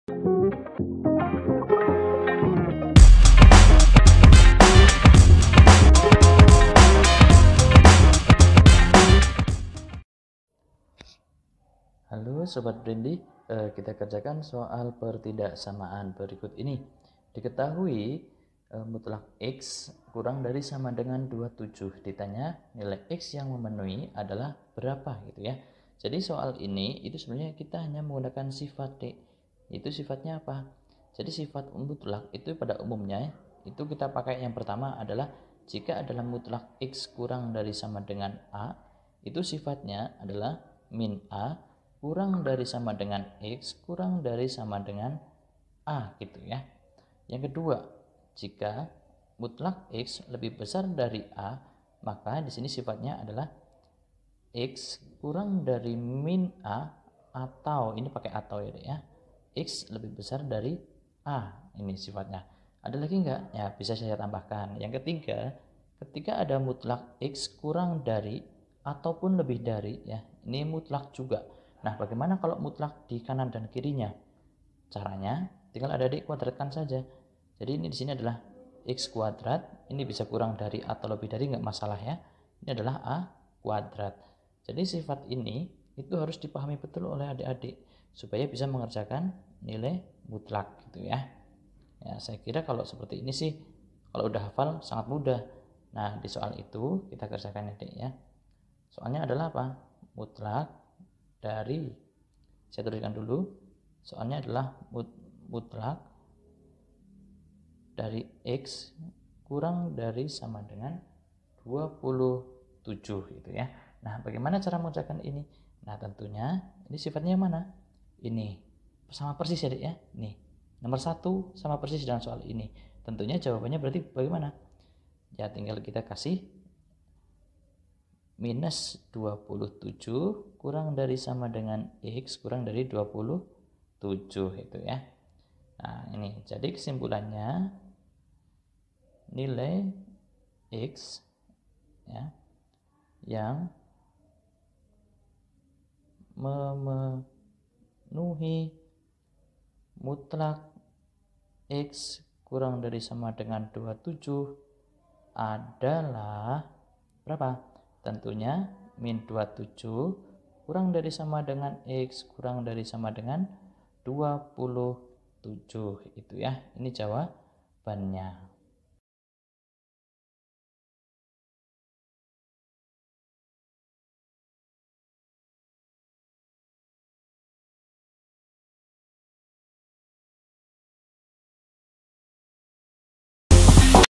|x|≤27 maka rentang nilai x adalah... | Pendalaman Buku | Kelas 10 | 1 | Matematika
✅ Dalam video ini kita akan membahas: |x|≤27 maka rentang nilai x adalah...
Tema:
Kelas: 10
Mata Pelajaran: Matematika
Bab: 1
Buku: Pendalaman Buku Teks Matematika SMA Kelas X Program Wajib 1A

👉 Buku yang telah tersedia:
{PLAYLIST_LINK}
Di mana kamu dapat menemukan kami?